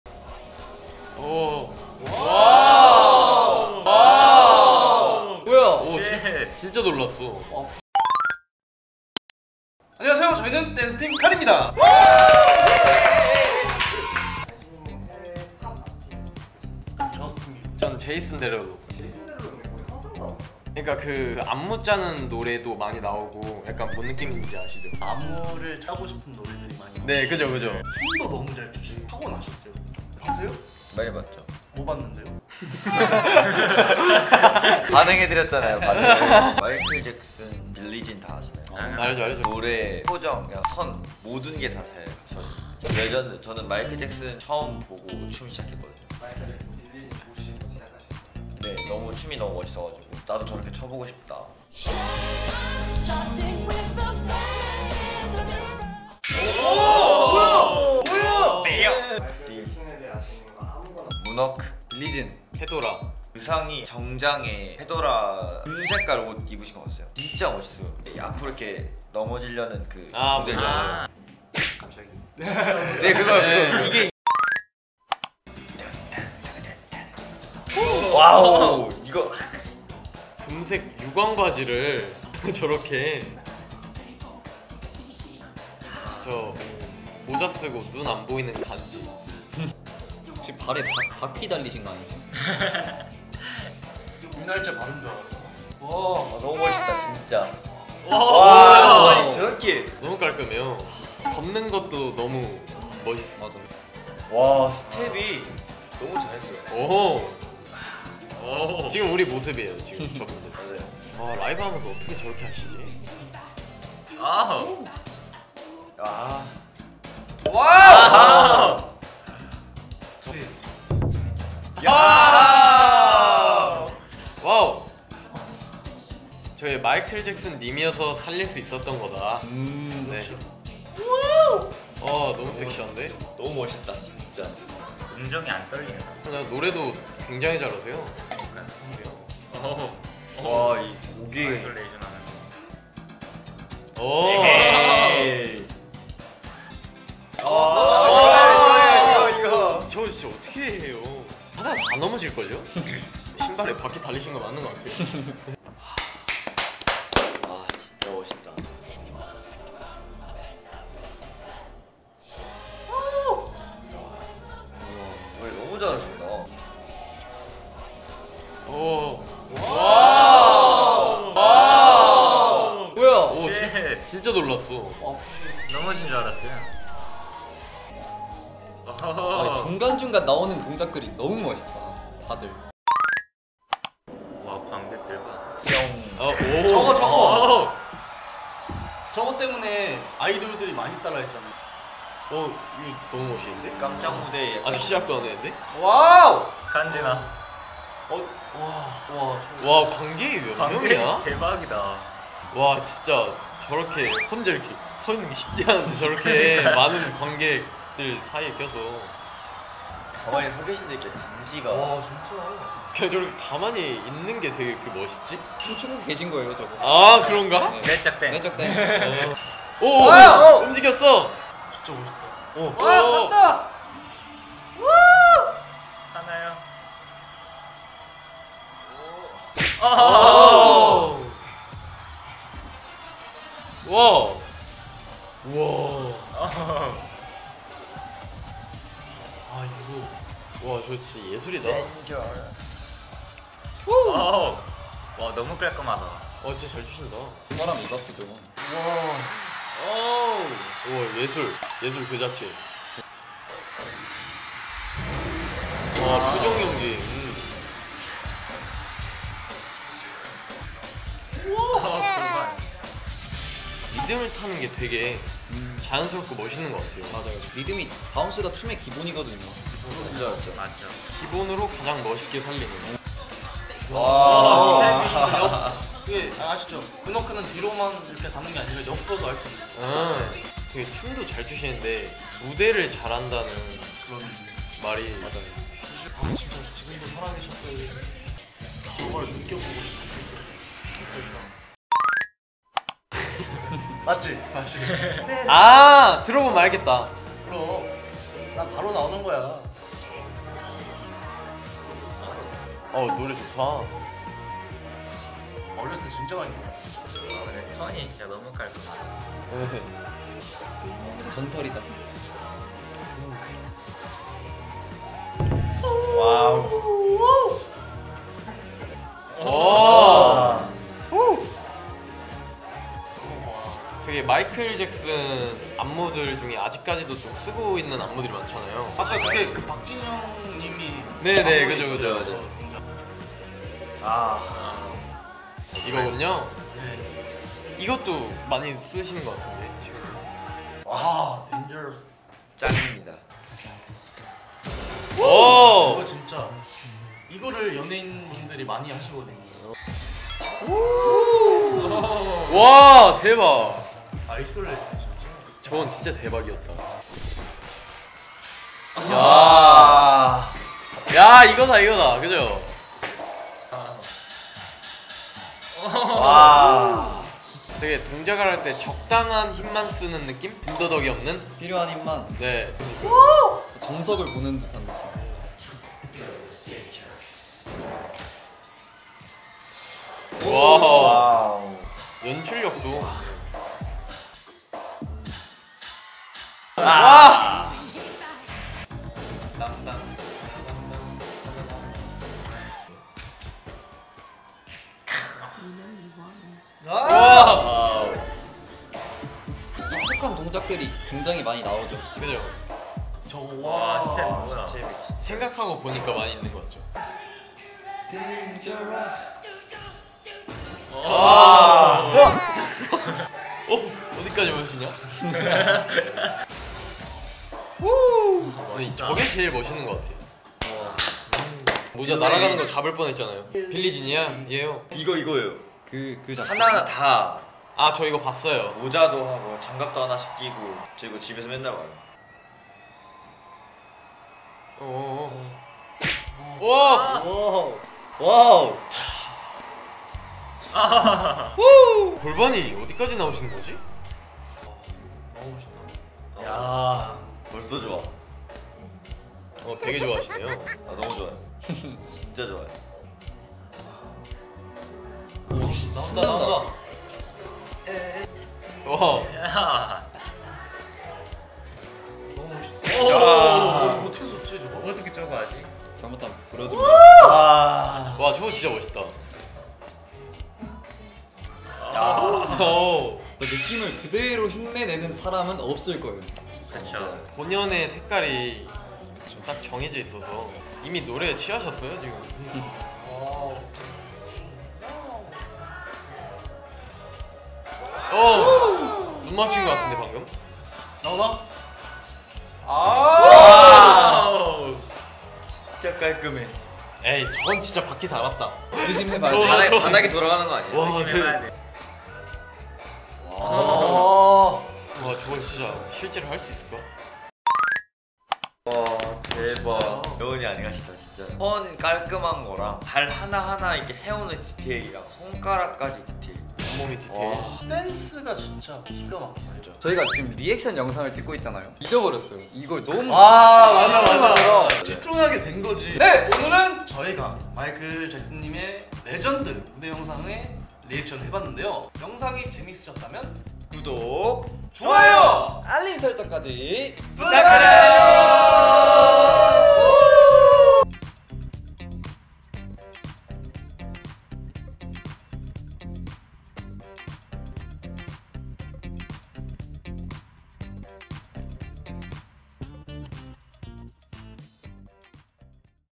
(ENG) 이정도라고..?! 실제 댄서들이 마이클 잭슨을 본다면? Korean Professional Dancers React to Michael Jackson Stages
레전드하면 항상 거론되는, 전설적인 팝의 황제 '마이클 잭슨'!
오늘은 국내 댄스팀 칼이 마이클 잭슨의 손꼽히는 무대들만 모아 리액션 해보았습니다!
지금 바로 보러가시죠!

☞추천하는 영상이나 도전해봤으면 하는 것들이 있으면 '댓글'로 남겨주세요!

☞밝고 표현력이 풍부한 10대,30대,50대 패널을 모집하고 있습니다!